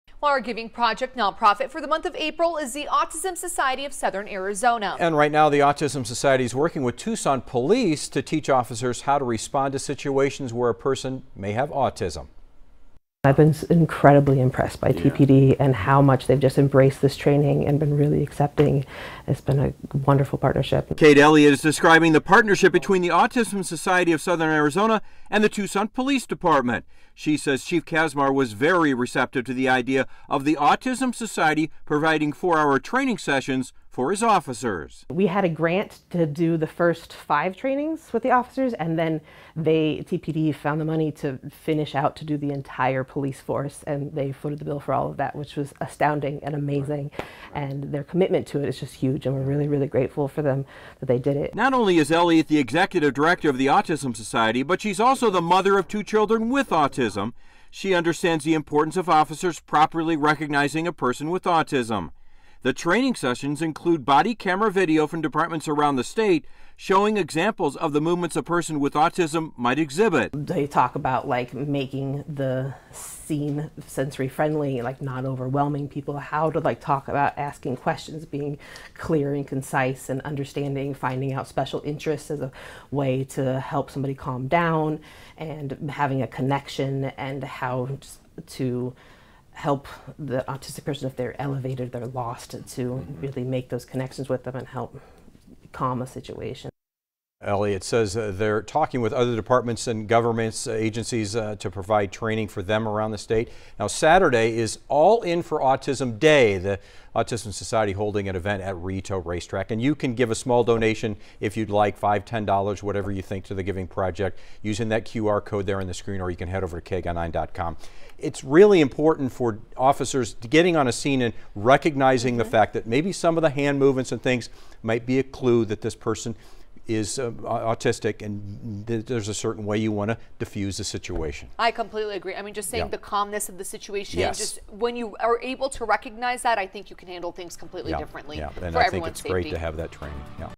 Special training helps Tucson officers recognize, respond to autism in field
The Autism Society of Southern Arizona has been working with the Tucson Police Department since September of 2022 to equip the entire police force with knowledge on ASD.